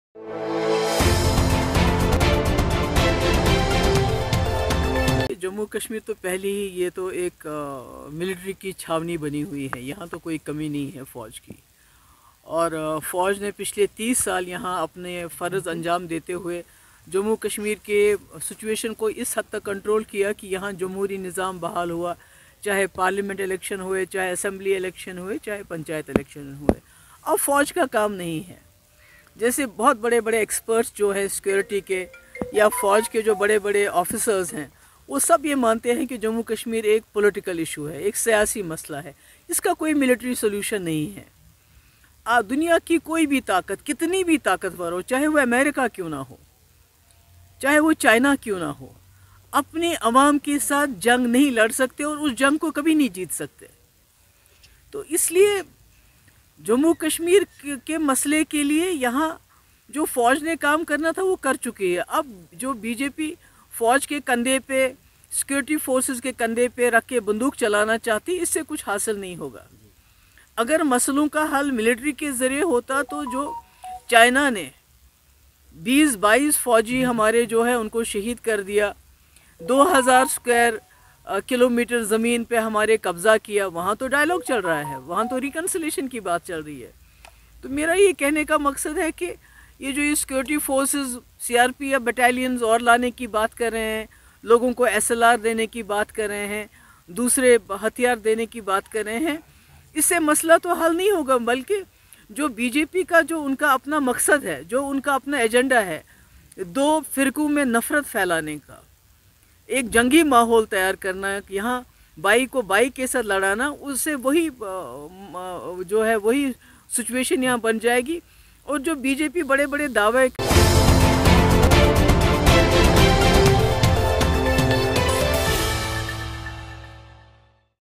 Modi Ji फौज के कंधे पर बंदूक रखकर चलाने से कश्मीर का मसला हल नहीं होगा......
फौज के कंधे पर बंदूक रखकर चलाने से कश्मीर का मसला हल नहीं होगा  महबूबा मुफ्ती